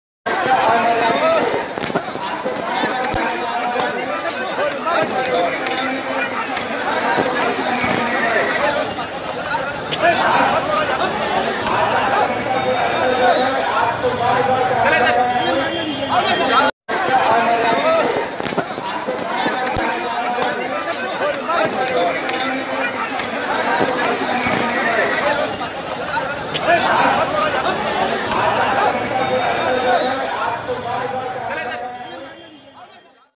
वाराणसी : प्रियंका के रोड शो, मोदी-मोदी का नारा लगा रहे लोगो को पुलिस ने जमकर पीटा...
वाराणसी : प्रियंका गांधी के रोड शो में लंका के बाद, गोदौलिया पर पिटे गए मोदी समर्थक, मोदी मोदी का नारा लगा रहे लोगो को पुलिस ने जमकर पीटा...